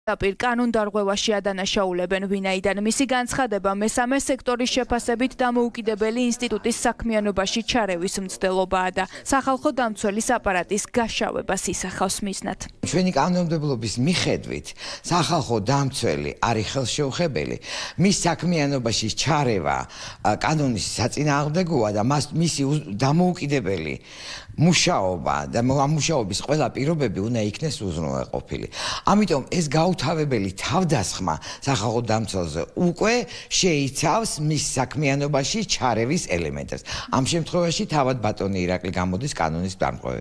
მთავარი, 21:00, 19/11/2021
ჩვენი კანონმდებლობის მიხედვით სახალხო დამცველი არის ხელშეუხებელი. მის საქმიანობაში ჩარევა კანონის საწინააღმდეგოა. მისი დამოუკიდებელი მუშაობა და ამ მუშაობის ყველა პირობები უნდა იქნეს უზრუნველყოფილი, ამიტომ ეს გაუთავებელი თავდასხმა სახალხო დამცველზე უკვე შეიცავს  მის საქმიანობაში ჩარევის ელემენტებს - ქეთევან ხუციშვილი